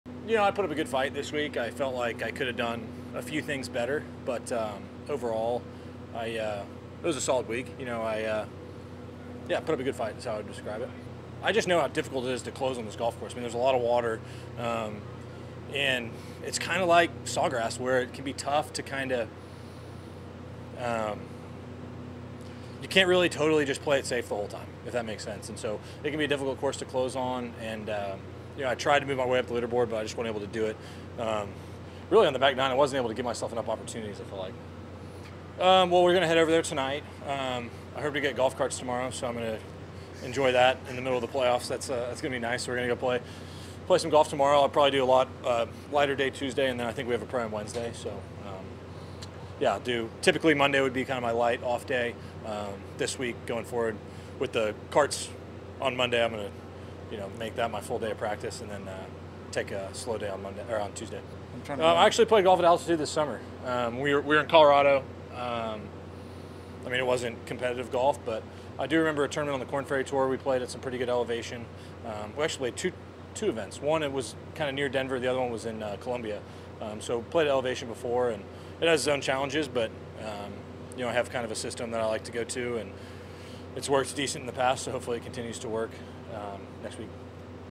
Scottie Scheffler Finishes 4th At St. Jude, Moving On To BMW Championship
Scottie Scheffler spoke to the media after finishing 4th at the FedEx St. Jude Championship, where he was asked:

- How would you assess the day?
- Were you aware of Hideki struggling for a stretch on the back 9?
- What are your plans looking towards the BMW?
- When was the last time you played golf at altitude?